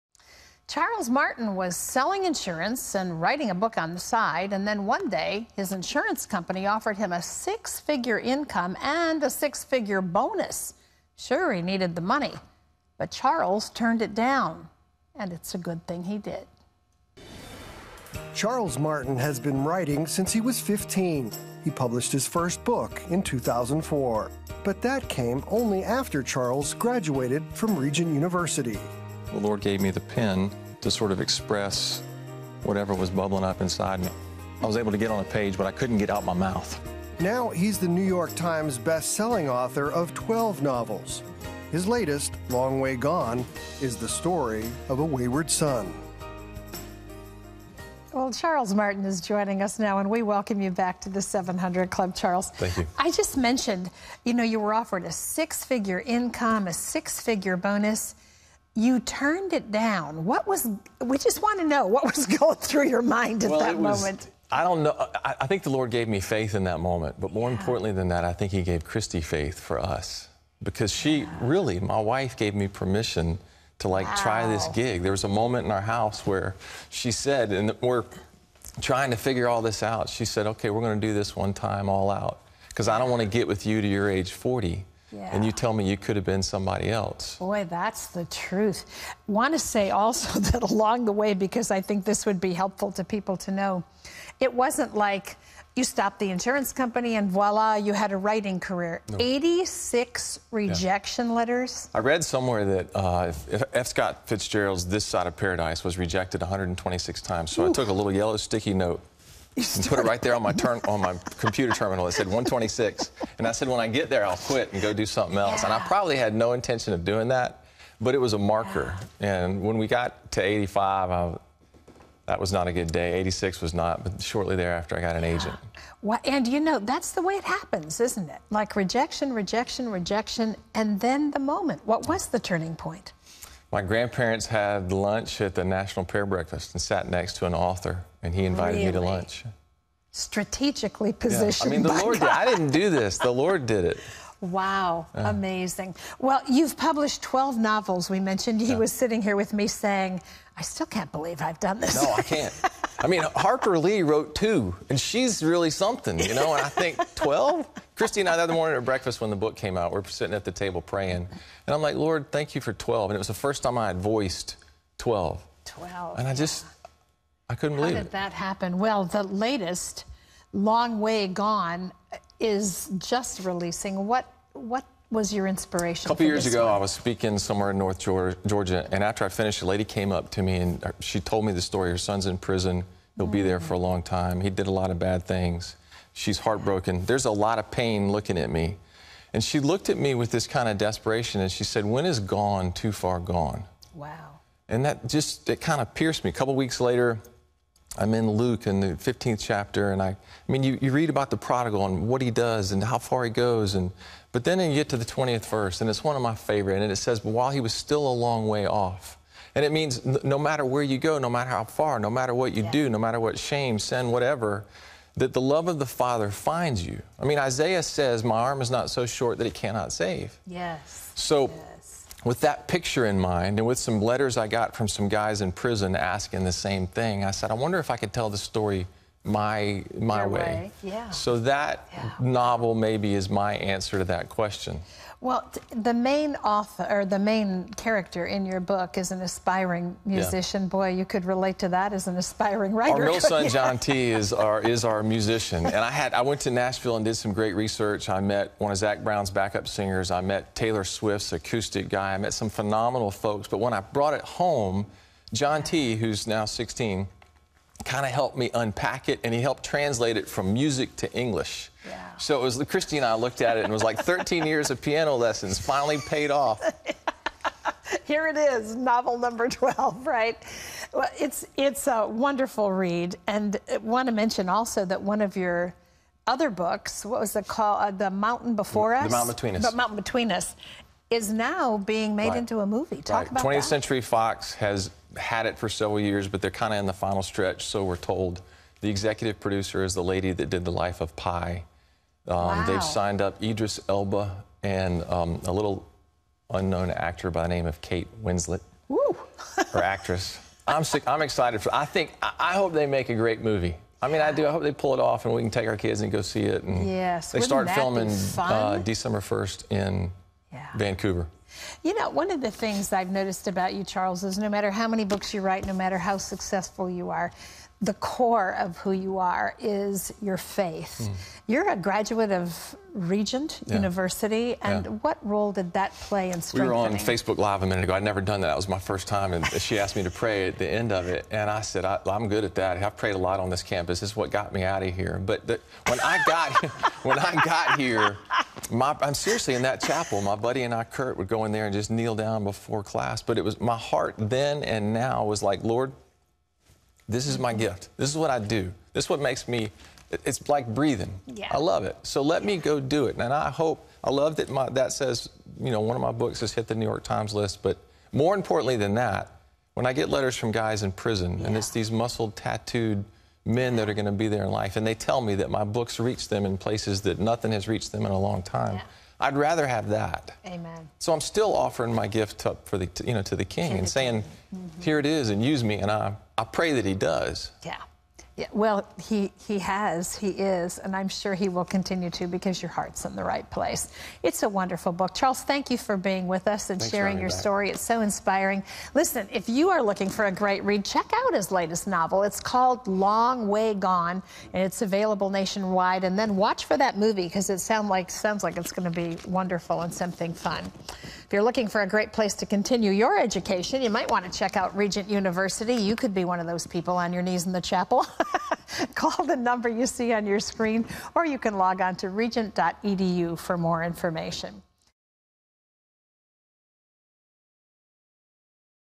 It's Not Too Late To Come Home
New York Times best-selling author, Charles Martin shares the story of the prodigal son for those wondering if it's too late to return home.